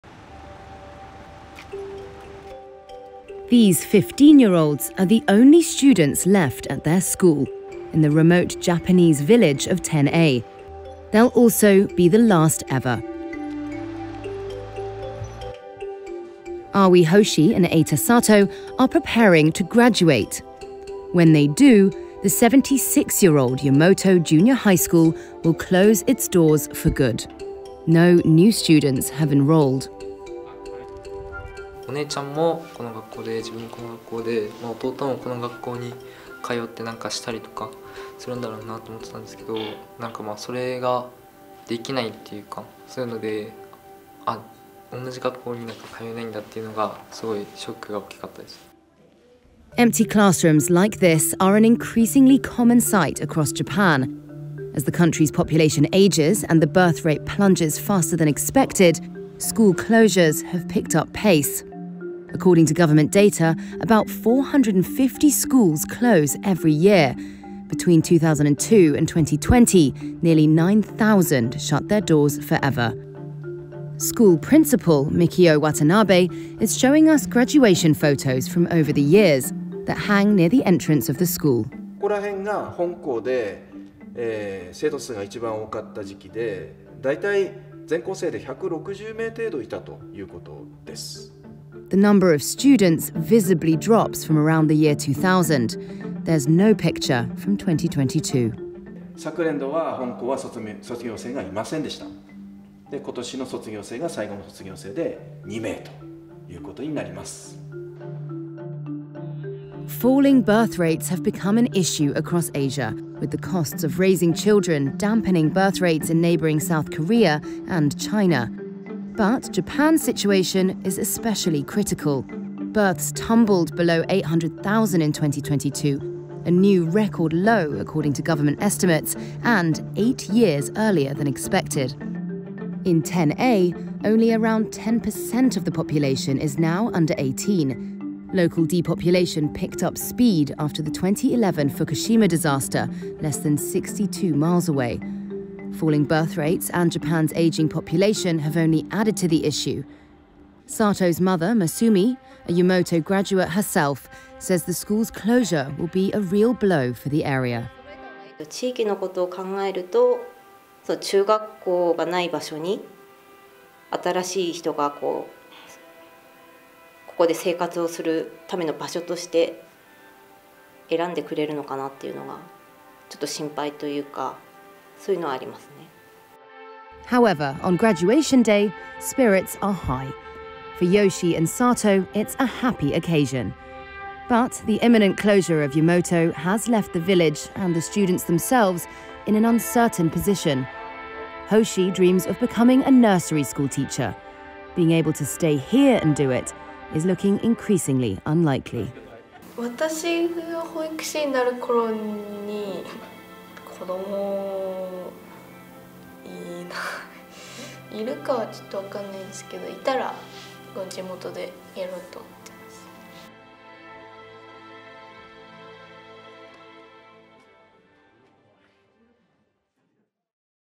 Japan school closes as the last two students graduate
Aoi Hoshi and Eita Sato, age 15, are the only students left at Yumoto Junior High School in Japan's remote mountain village of Ten-ei, in Fukushima Prefecture.